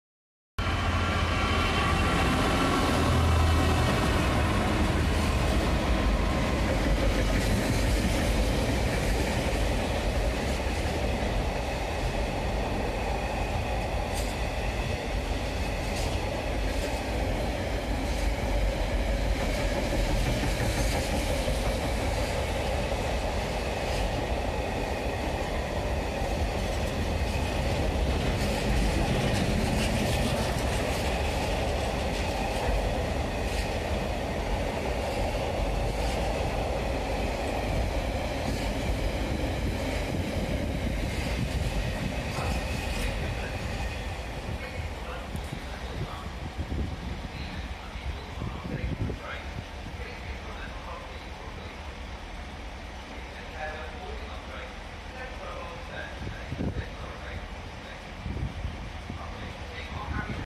66177 EWS
66177 EWS passing Lincoln working Wakefield to Felixstowe